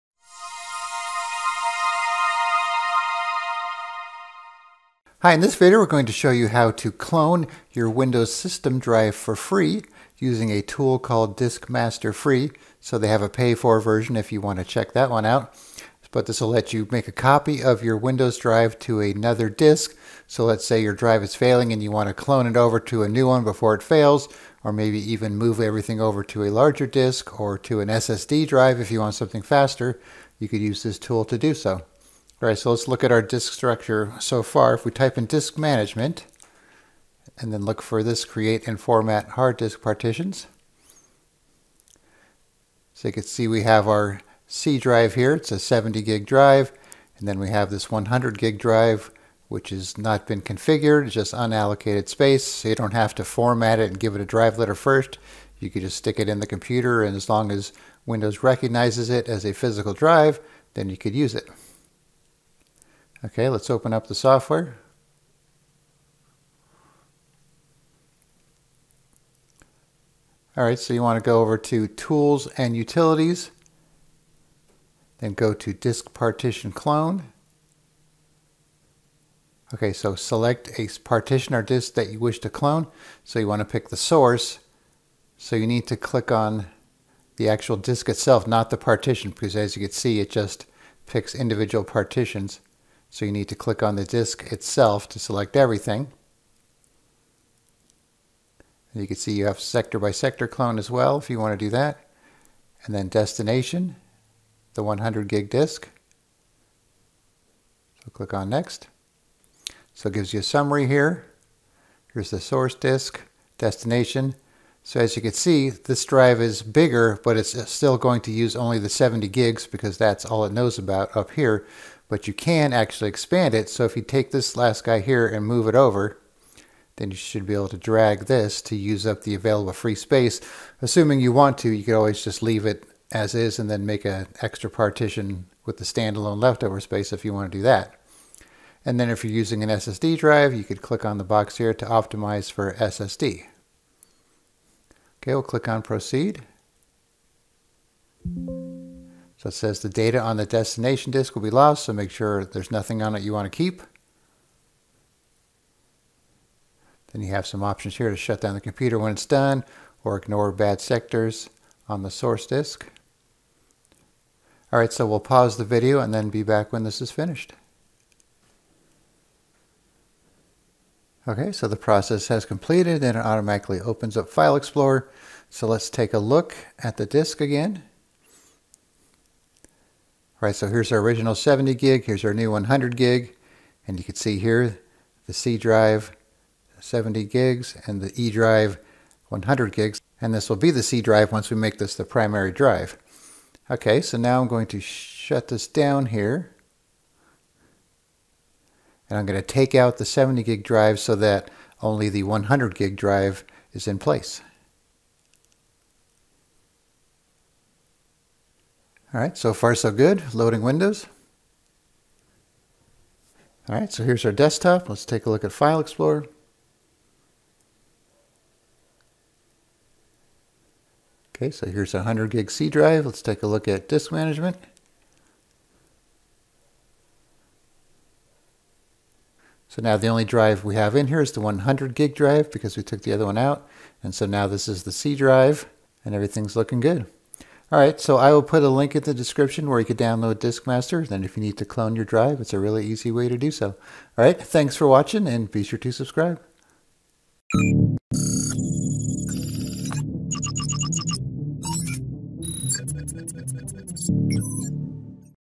How to Clone Your Windows \ System Drive Using Disk Master Free
If you have the need to clone your Windows\System drive to use with a new or larger drive you can easily clone it using the free Disk Master Free software. This also comes in handy if your hard drive is failing and you want to copy Windows along with your programs and files over to a new drive.

Here is a link to download the Disk Master Free software.